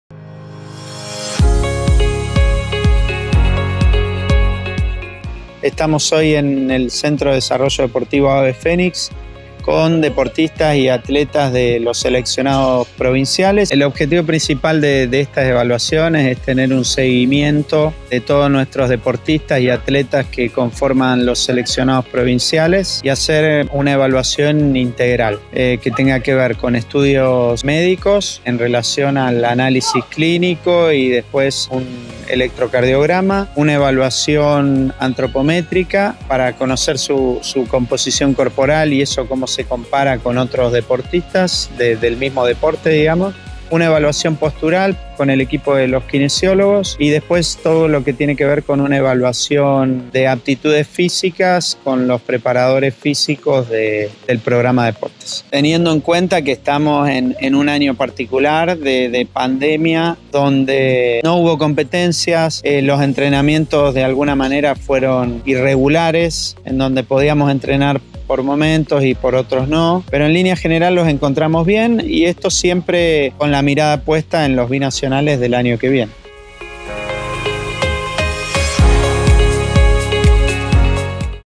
EVALUACIONES INTEGRALES A LAS SELECCIONES PROVINCIALES
Están a cargo de los médicos, kinesiólogos, nutricionistas y preparadores físicos del Programa Deportes. Este martes y miércoles fue el turno de las selecciones de natación y atletismo, que ya se preparan para los Juegos Binacionales 2021.

Entre las pruebas médicas se destacan una evaluación clínica general, de anamnesis deportiva (historial clínico – deportivo), de talla, auscultación y un electrocardiograma. En cuanto a lo kinesiológico, los profesionales se enfocaron en evaluaciones posturo – articular, estática y dinámica de la columna, test de Lunge (movilidad de tobillo), entre otras. La nutricionista se encargó de una anamnesis nutricional y antropometría (para conocer la composición y distribución de la masa corporal, grasa y ósea), mientras que los preparadores físicos se enfocaron en pruebas de saltabilidad y sobre métodos y técnicas del desarrollo de la fuerza en el período de entrenamiento.

Las evaluaciones continuarán el próximo martes y miércoles con las selecciones de taekwondo y ciclismo, respectivamente, mientras que el 1º de diciembre será el turno de los deportistas con proyección panamericana.